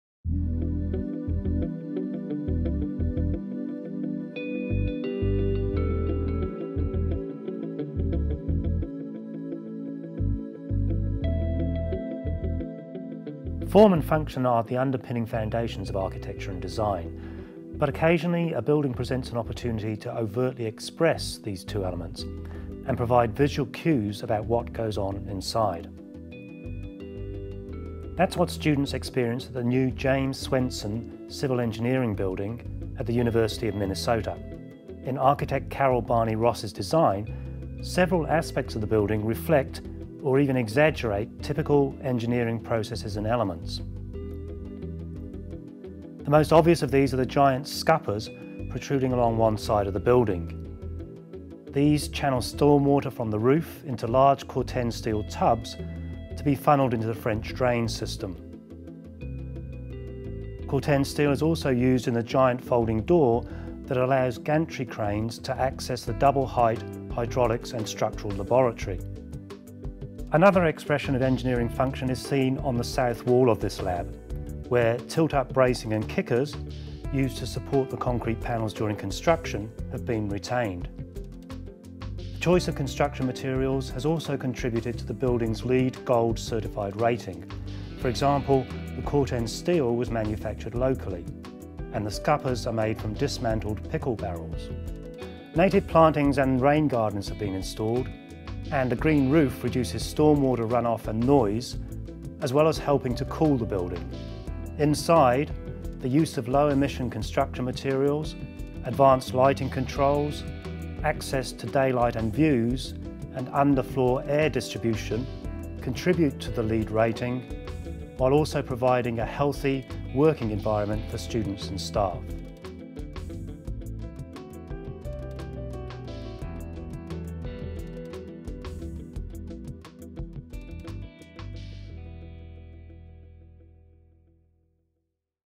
James Swenson Civil Engineering building at the University of Minnesota design technology
Read this story at Trendsideas

Trends Commercial Design Perspectives - Healthy expression.
Trends Editorial Director Paul Taylor looks at a university building that embodies the studies that happen inside.